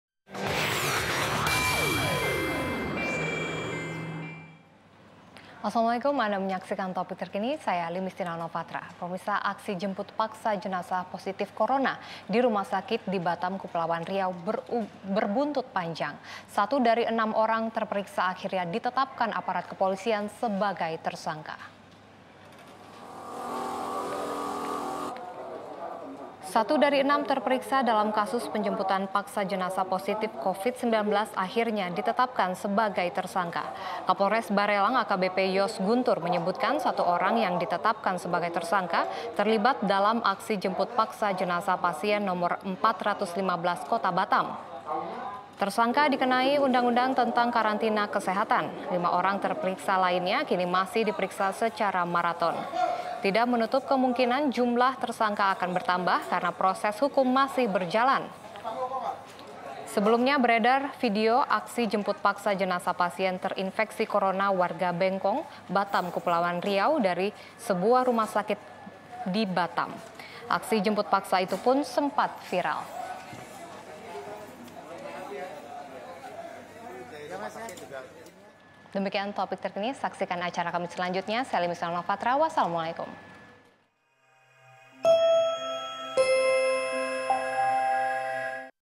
Aksi Jemput Paksa Jenazah Pasien Terinfeksi Corona
Aksi Jemput Paksa Jenazah Positif Corona Di Rumah Sakit Dibatam Kepulauan Riau Berbuntut Panjang, Satu Dari Enam Orang Terperiksa Akhirnya Ditetapkan Aparat Kepolisian Sebagai Tersangka